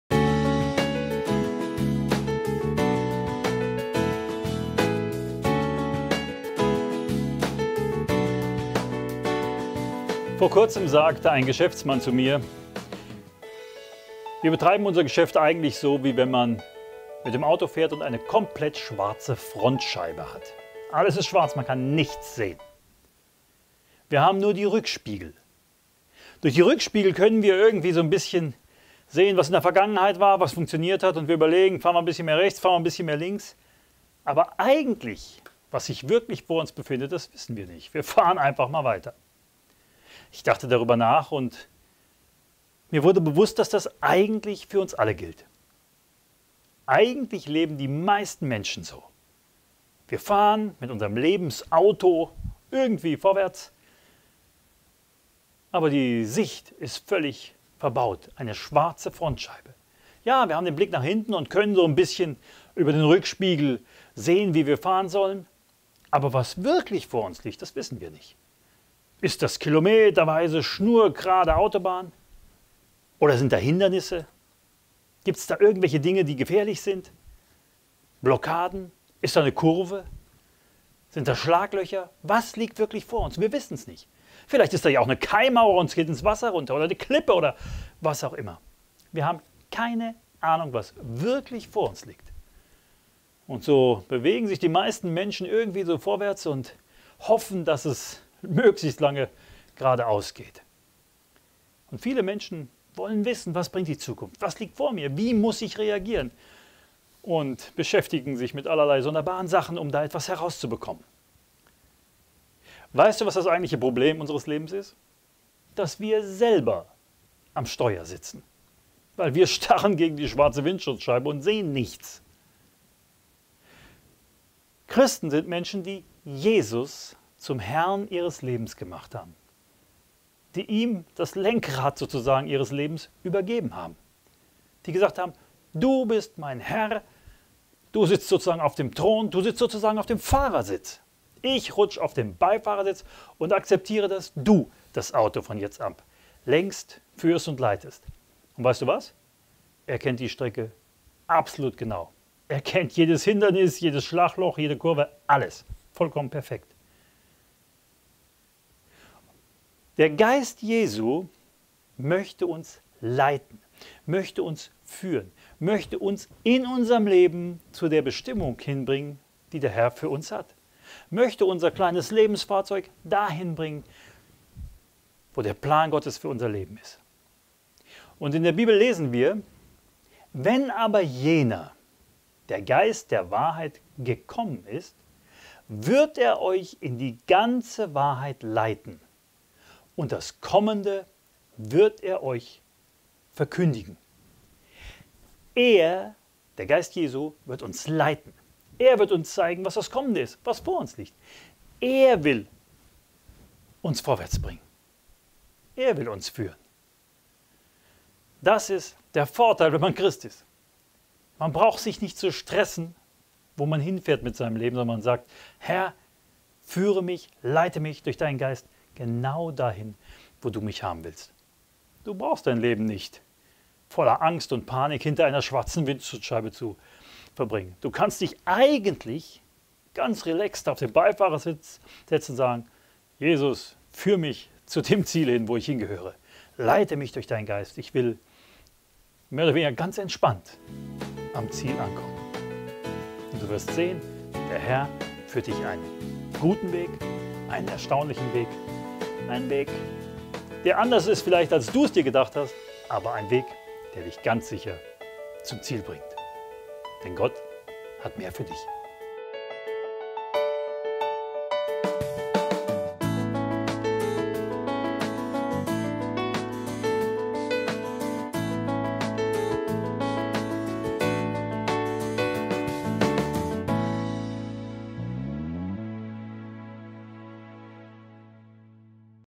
Mit schwarzer Frontscheibe
Unser Leben ist wie Autofahren mit schwarzer Frontscheibe. Man kann nicht sehen, was wirklich vor einem liegt, sondern muss mit dem Blick in die Rückspiegel versuchen, das Auto irgendwie zu lenken. Manch einer verbingt sein Leben voller Angst und Panik hinter dieser schwarzen Scheibe. Doch es gibt eine Alternative!

Es ist tatsächlich so: Die Sicht unseres Lebens ist verbaut. Wir wissen nicht, was vor uns liegt und fahren mit unserem kleinen Lebensfahrzeug irgendwie voran, in der Hoffnung, dass es möglichst lange gutgeht.

Das eigentliche Problem ist, dass du es bist, der am Steuer sitzt. Was du brauchst, ist jemand, der die Strecke kennt und den Überblick hat.
Räume deshalb deinen Platz und überlasse Jesus Christus das Lenkrad deines Lebens. Er kennt die vor dir liegende Strecke genau. Er kennt das gefährliche Gelände, die Hindernisse und Kurven. Übergib ihm den Fahrersitz und rutsche einfach auf den Beifahrersitz. Es wird zu deinem größten Segen sein.

Wenn aber jener, der Geist der Wahrheit, gekommen ist, wird er euch in die ganze Wahrheit leiten ... und das Kommende wird er euch verkündigen.
Johannes 16,13

Der Beistand aber, der Heilige Geist, den der Vater senden wird in meinem Namen, der wird euch alles lehren
Johannes 14,26

Leite mich in deiner Wahrheit und lehre mich, denn du bist der Gott meines Heils; auf dich harre ich den ganzen Tag.
Psalm 25,5